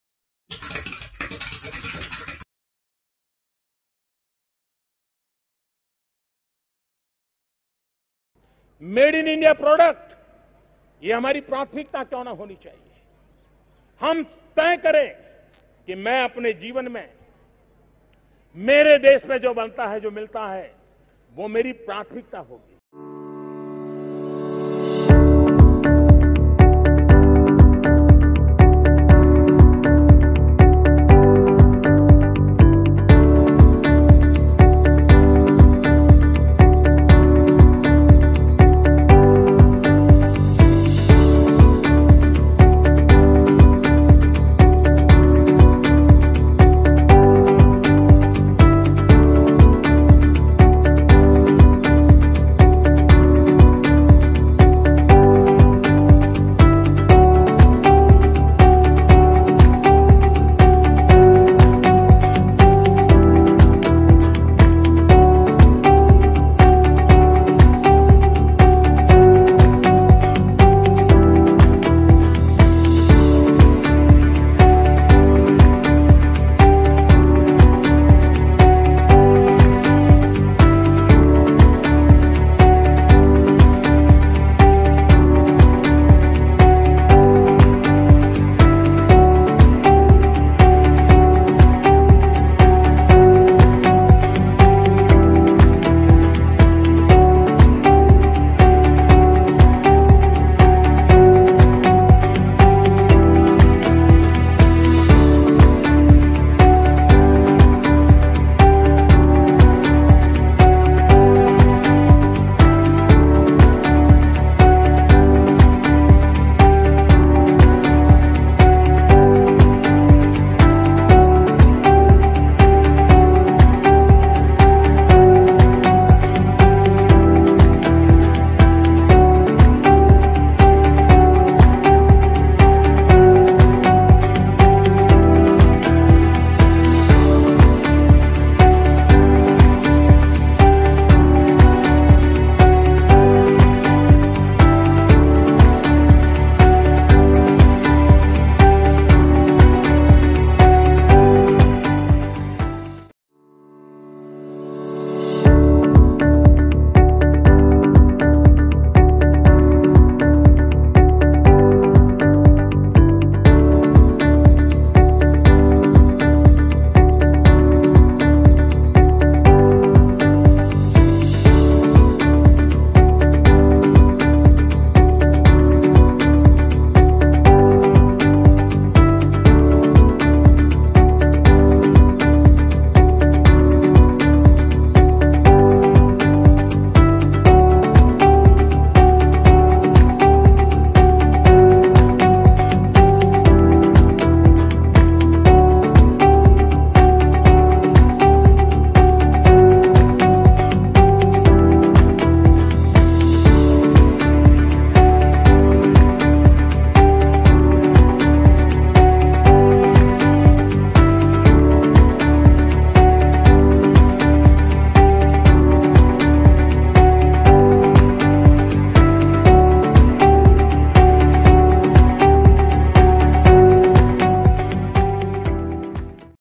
Secureye - India's Leading Player in Security Industry | Manufacturing Facility at New Delhi, India
Secureye is India's Leading Player in the Security Industry with a Manufacturing Unit at the heart of Delhi, Pushing the MakeInIndia Initiative of Govt of India.

We also pledge to become AatmaNirbharBharat with state-of-the-art technology in the Manufacturing of Security Products like CCTV Cameras, Biometric Machine, Access Controls, and many more.